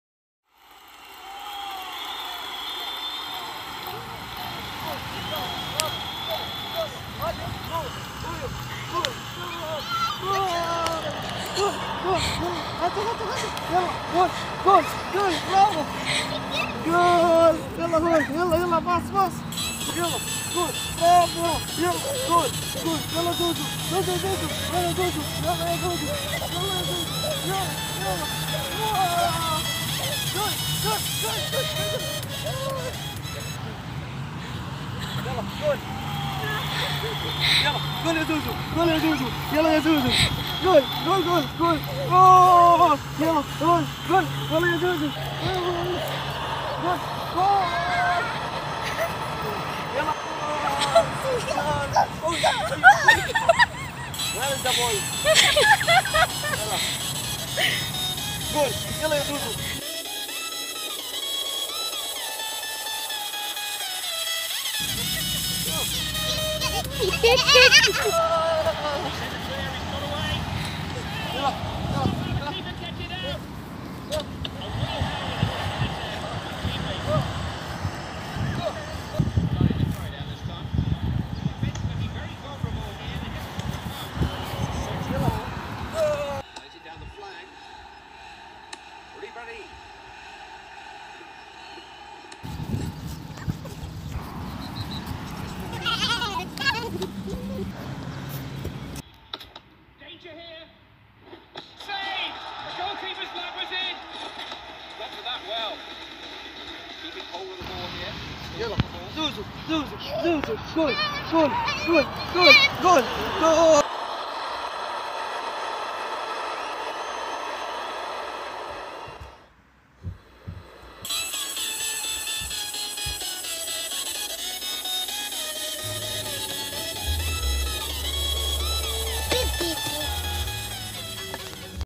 Watch: Playing football with Dad | Time spent with Family is worth every second
"No amount of money can takes place the time we spent with our Family".
👶💕Baby Zain's first time to play football in the field..Family Bonding.